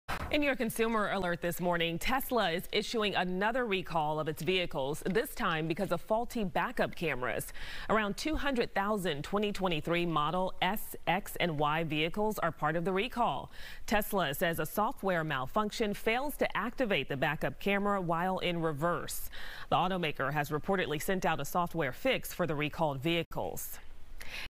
Tesla recalls 2020-2023 models for faulty backup cameras
Tesla is recalling around 200,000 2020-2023 Model S, X, and Y vehicles due to a software malfunction that fails to activate the backup camera when in reverse.